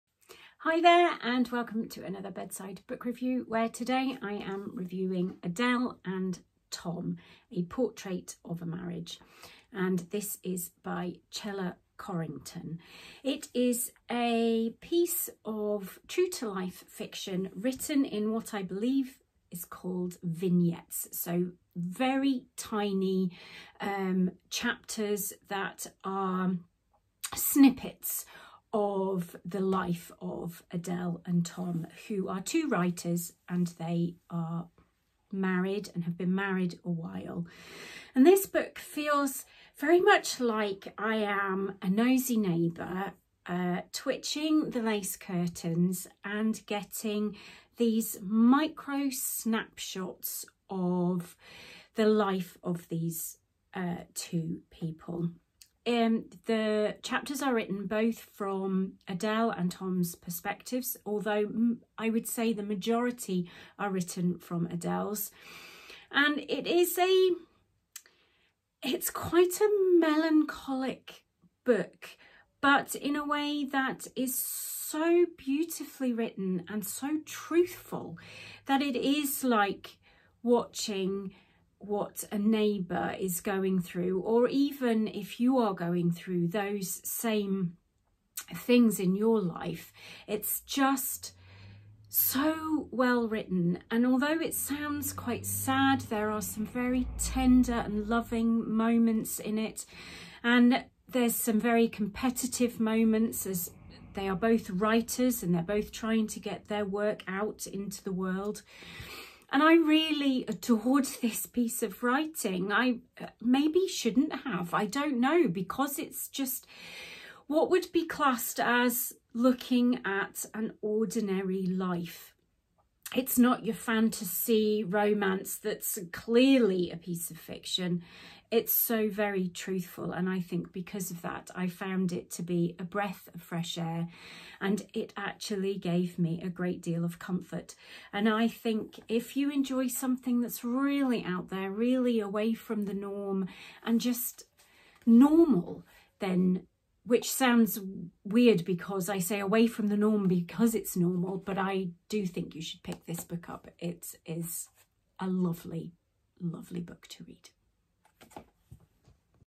Adele and Tom: The Portrait of a Marriage by Chella Courington
Short review of a true to life fiction. Written in vignettes this is a snap shot of the lives of Adele and Tom.